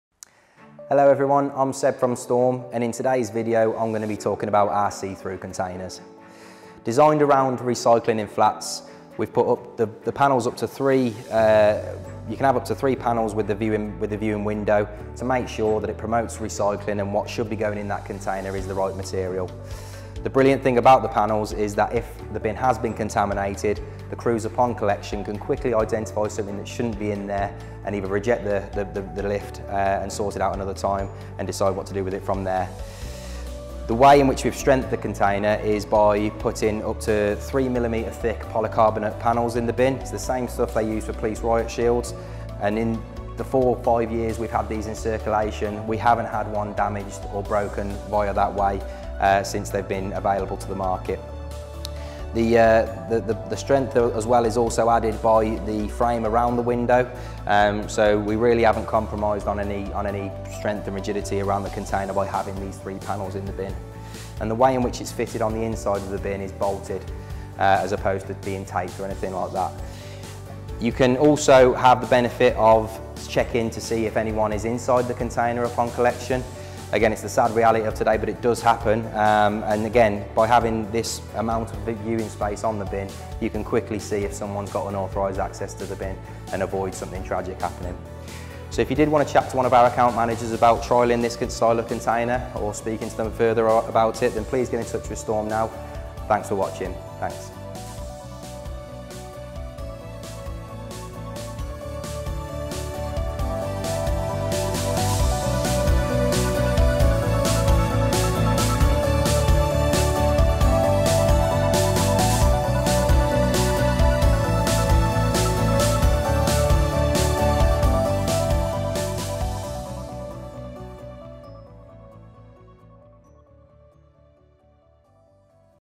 Product Demonstration | See Through Bin
The See Through 1100 litre container is designed for the storage and collection of dry mixed recyclables. Available with front, side or a combination of both viewing panel types.
All standard features of the EN840 container, Strong thick poly-carbonate clear plastic viewing windows, Extremely heavy steel fabrication to protect viewing windows from damage, Prevents rejection of recycled materials by processing mills, Enables unauthorised persons who may be sleeping in the container to be seen, preventing injury or even death.